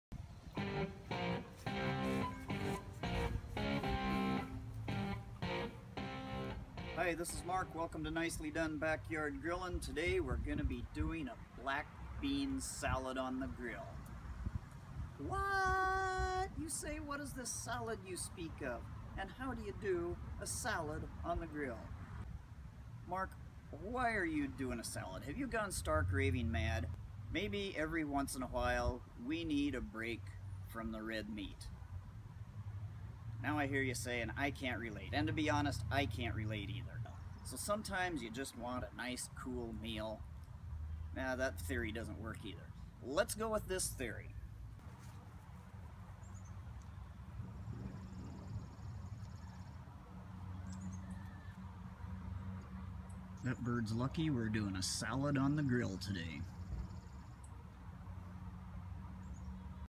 Nisley Done Backyard Grillin' Episode 4 Trailer
Really? A black bean salad on the grill? Have we gone stark raving mad? And what is that yard bird doing wandering around loose in the city limits? Has the world gone stark raving mad?